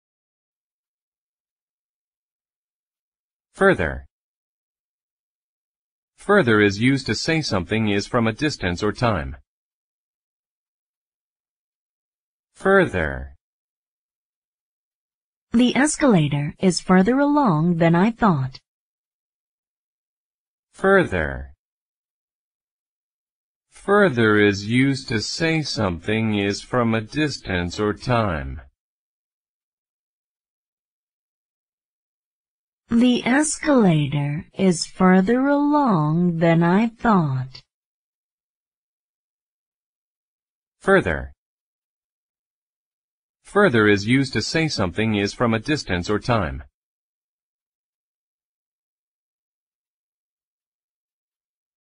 Further : B1 level english vocabulary lesson
In this english video we will see what the word "Further" means. It's an English word from B1 level vocabulary list.

The word is "Further", meaning is: "Further is used to say something is from a distance or time." and example is: "The escalator is further along than I thought.".